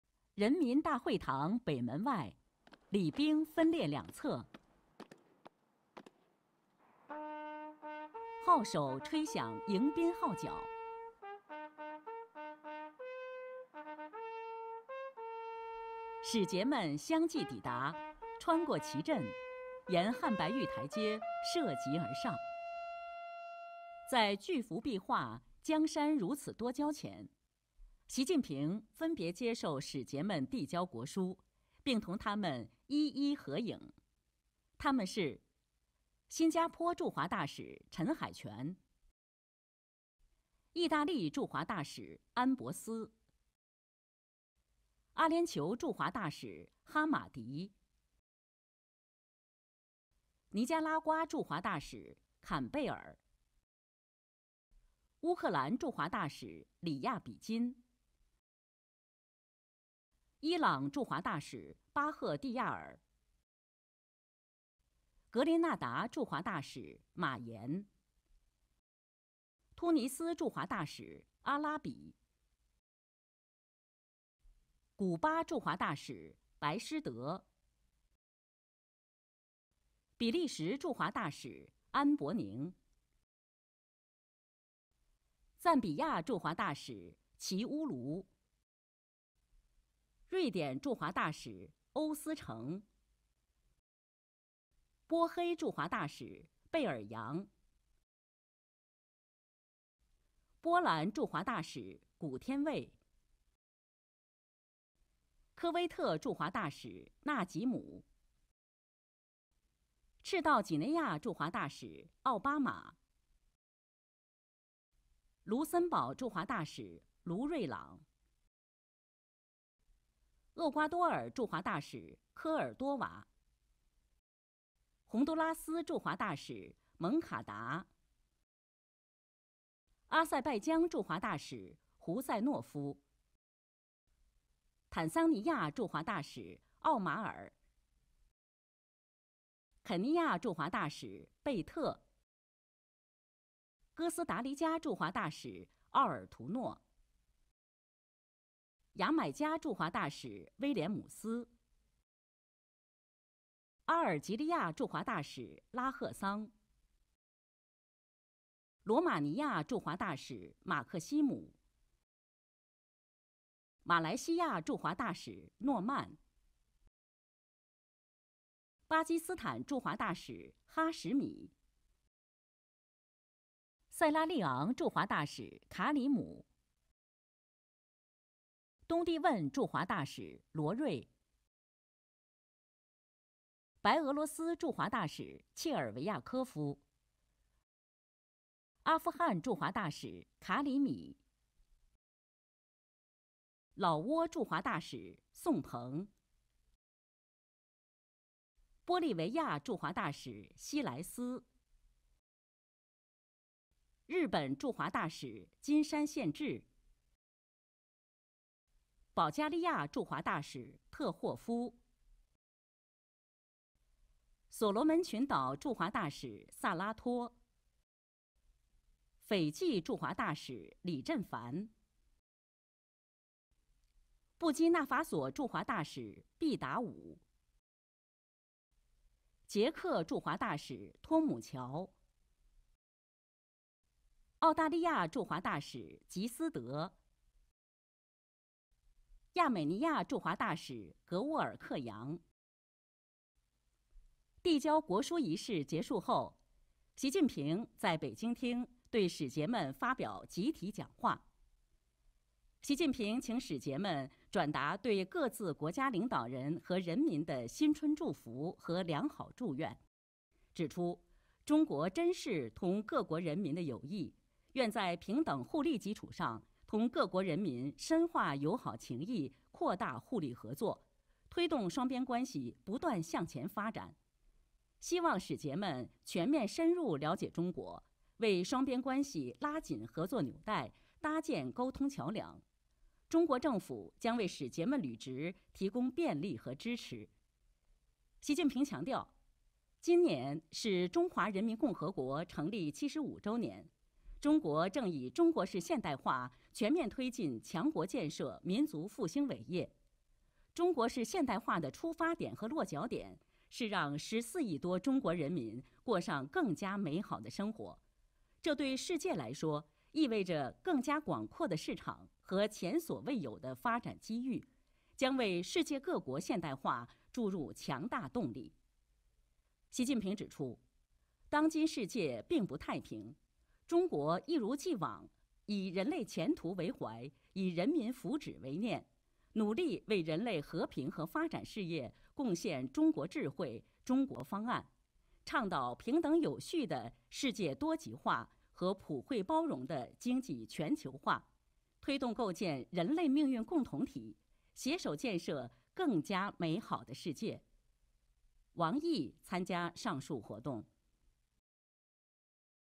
Xi Jinping receives credentials of 42 ambassadors to China
Chinese President Xi Jinping received the credentials of 42 ambassadors to China at the Great Hall of the People in Beijing on Tuesday afternoon.

The ambassadors are Tan Hai Chuan from Singapore, Massimo Ambrosetti from Italy, Hussain Alhammadi from the United Arab Emirates, Michael Campbell from Nicaragua, Pavlo Riabikin from Ukraine, Mohsen Bakhtiar from Iran, Ian J. Marshall from Grenada, Adel Elarbi from Tunisia, Alberto Blanco Silva from Cuba, Bruno Angelet from Belgium, Iven Zyuulu from Zambia, Per Augustsson from Sweden, Sinisa Berjan from Bosnia and Herzegovina, Jakub Kumoch from Poland, Jasem Ibrahem Alnajem from Kuwait, Mauricio Epkua Obama from Equatorial Guinea, Roland Reiland from Luxembourg, Maria Soledad Cordova from Ecuador, Salvador Moncada from Honduras, Bunyad Huseynov from Azerbaijan, Khamis M Omar from Tanzania, Willy Bett from Kenya, Alfredo Ortuño Victory from Costa Rica, Arthur Williams from Jamaica, Kaid Slimane Lahcene from Algeria, Dan-Horia Maxim from Romania, Norman Bin Muhamad from Malaysia, Khalil Ur Rahman Hashimi from Pakistan, Abu Bakarr Karim from Sierra Leone, Loro Da Silva Horta from Timor-Leste, Charviakou Aliaksandr from Belarus, Asadullah Bilal Karimi from Afghanistan, Somphone Sichaleune from Laos, Hugo Siles from Bolivia, Kanasugi Kenji from Japan, Andrey Tehov from Bulgaria, Barrett Salato from Solomon Islands, Robert Lee from Fiji, Daouda Bitie from Burkina Faso, Martin Tomco from the Czech Republic, Scott Dewar from Australia, and Vahe Gevorgyan from Armenia.

After the ceremony, Xi delivered a speech to the ambassadors.

Xi asked the ambassadors to convey his cordial greetings and best wishes to their state leaders and the peoples. Noting that China cherishes friendship with people of other countries, Xi said China is willing to deepen friendship and expand mutually beneficial cooperation with people of other countries on the basis of equality and mutual benefit, and to push forward bilateral relations.

It is hoped that ambassadors will have a comprehensive and in-depth understanding of China and serve to strengthen the bond of cooperation and build a bridge of communication for bilateral relations. The Chinese government will provide support and convenience for ambassadors to perform their duties, Xi said.

"This year marks the 75th anniversary of the founding of the People's Republic of China," Xi stressed, adding that China is advancing the building of a strong and prosperous country and national rejuvenation in all respects through Chinese modernization. The mission and objective of Chinese modernization are to enable the over 1.4 billion Chinese people to live a better life. For the world, this means a broader market and unprecedented development opportunities, which will inject strong impetus into the modernization of countries around the world.

Xi said the world today is not peaceful. With the future of mankind and the well-being of its people in mind, China, as always, endeavours to contribute its wisdom and solutions to the cause of peace and development of mankind, advocate an equal and orderly multipolar world and a universally beneficial and inclusive economic globalization, promote the building of a community with a shared future for humanity, and work together to build a better world.

Wang Yi was present at the events.